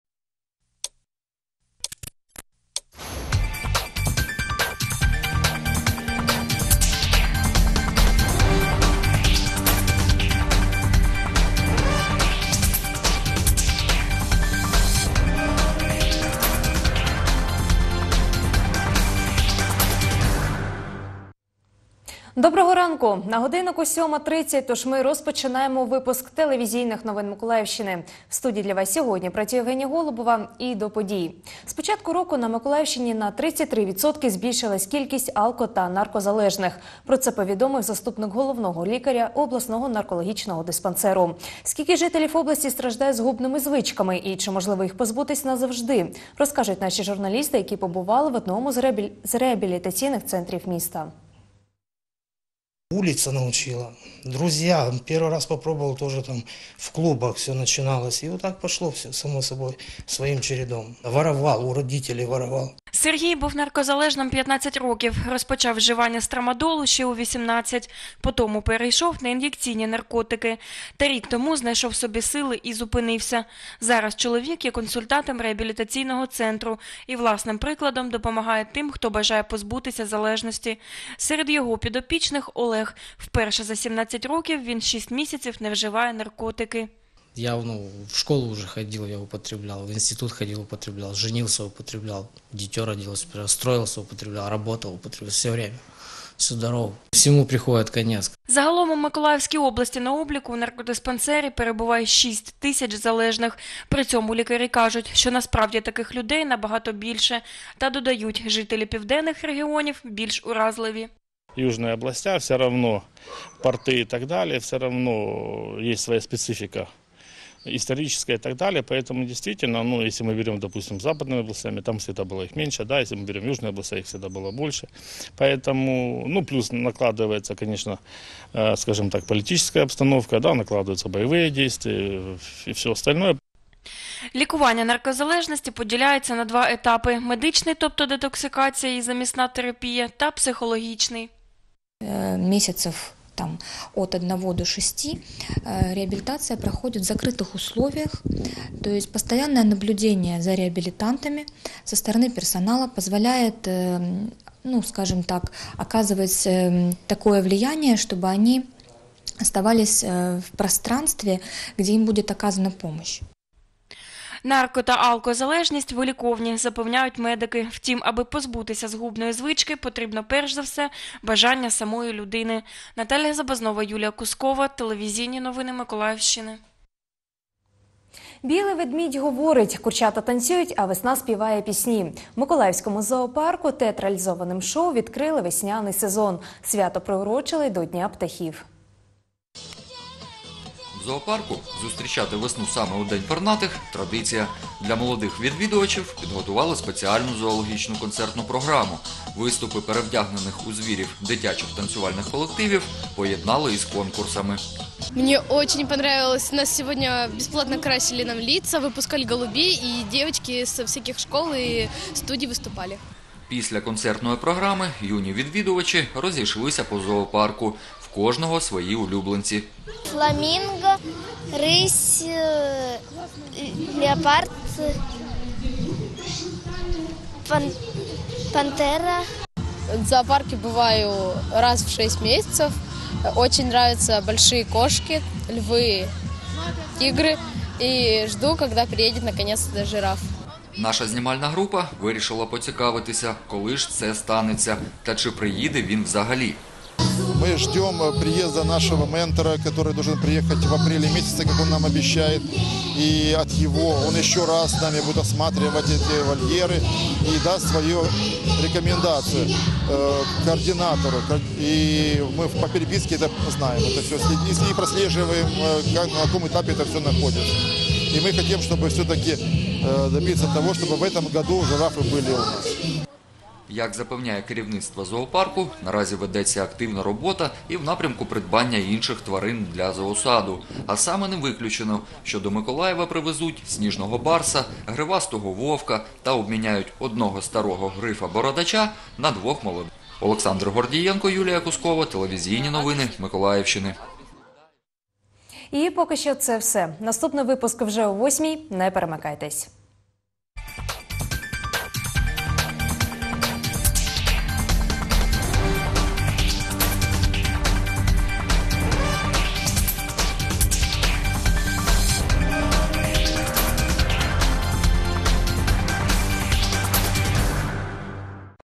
Телевізійні новини Миколаївщини - 07:30 - (02.04.2018)
Інформаційна картина дня (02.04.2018) поглядом кореспондентів телеканалу “Миколаїв” (випуск новин — 07:30):
- на Миколаївщині на 33 % збільшилась кількість алко- та наркозалежних;
- День птахів у Миколаївському зоопарку.